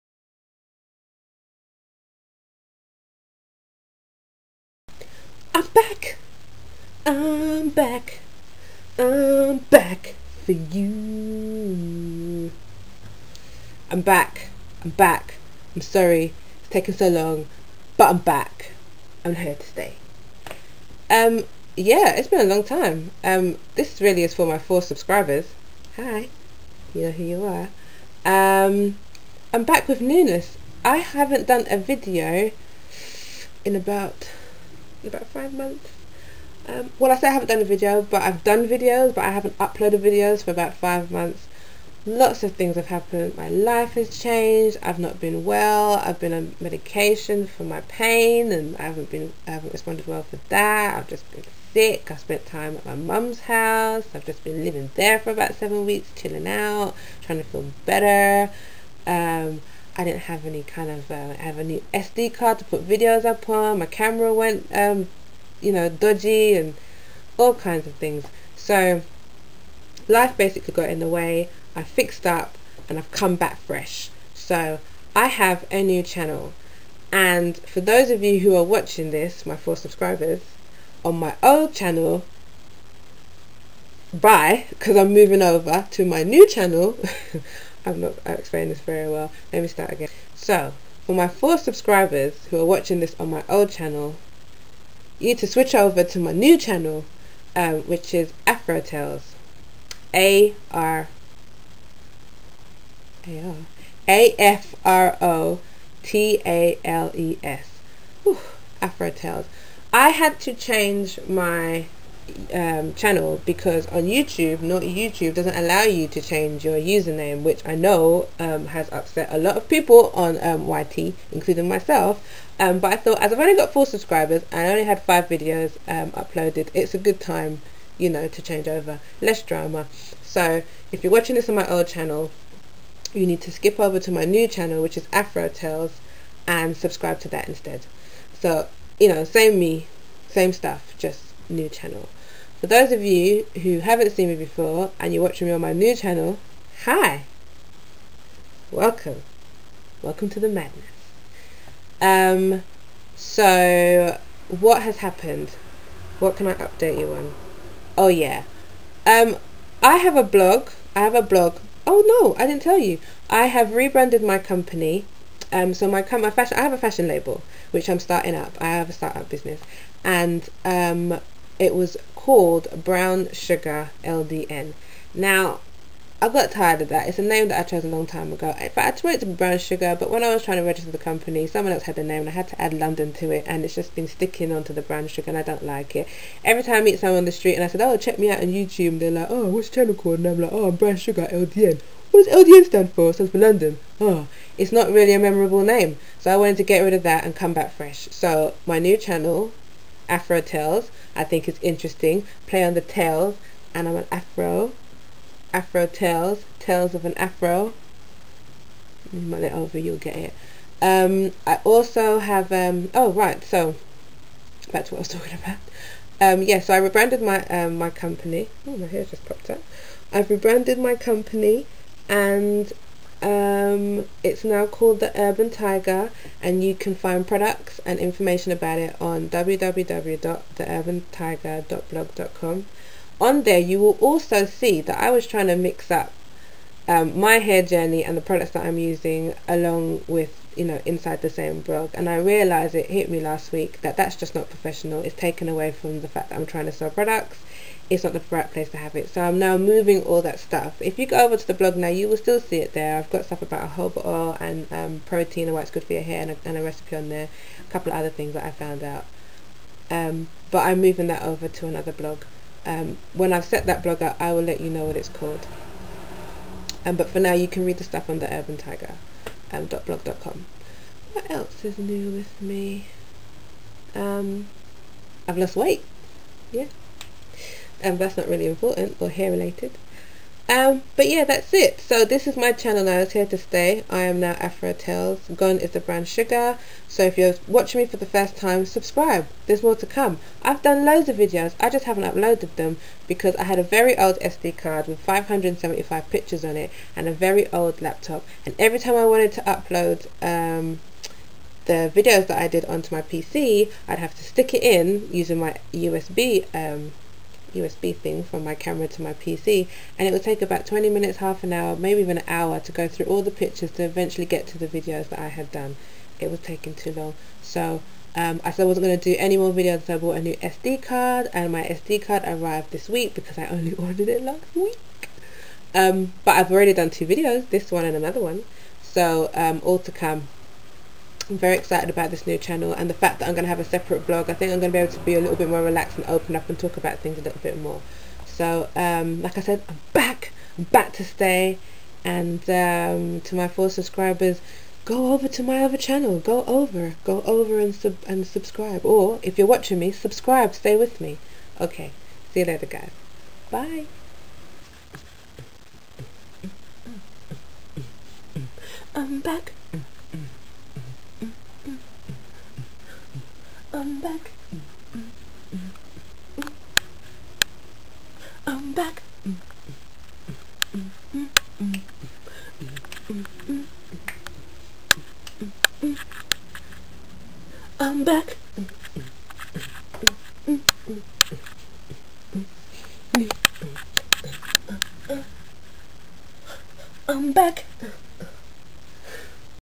#1 I'm back on the Tube!!!
I'm back you guys -Fresh, Improved and with a new channel. I've switched up channels ( from BrownsugarLdn to Afro Tales) because  I wanted a fab new username.